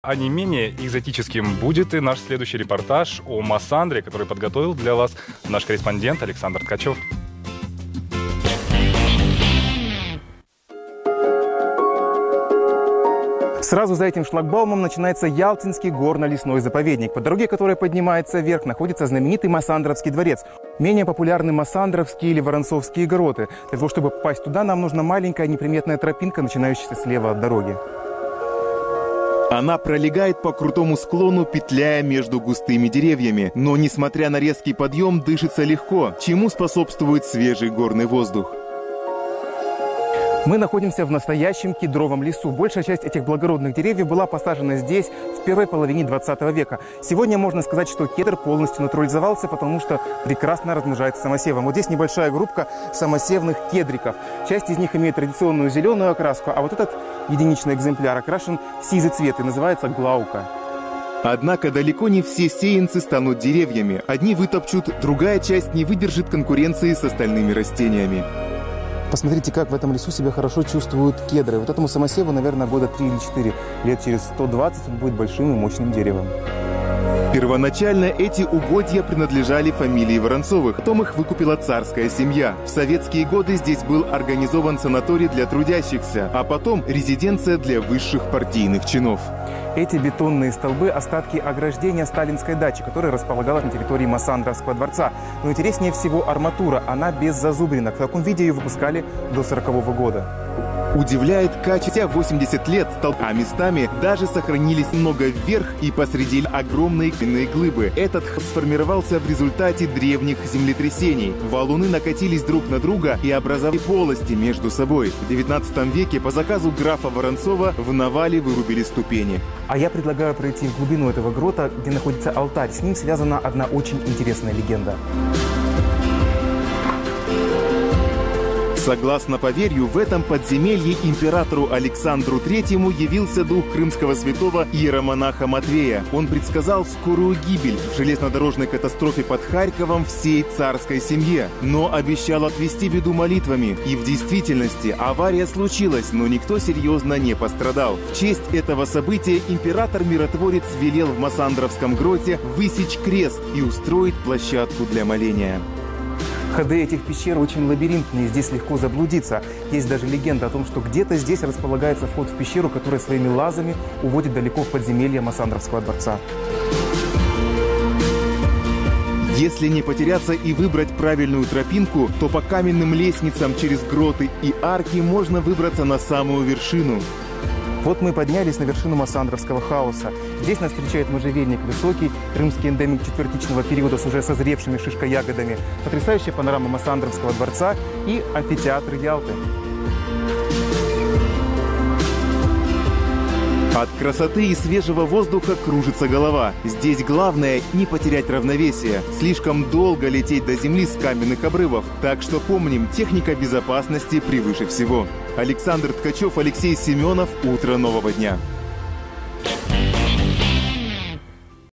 Массандровские гроты - уникальные геологические памятники на Никитской Яйле
Массандровские гроты - уникальные геологические памятники на Никитской Яйле. Подземелья хранят много тайн.  К тайнам подземелий прикоснулся Александр Ткачёв.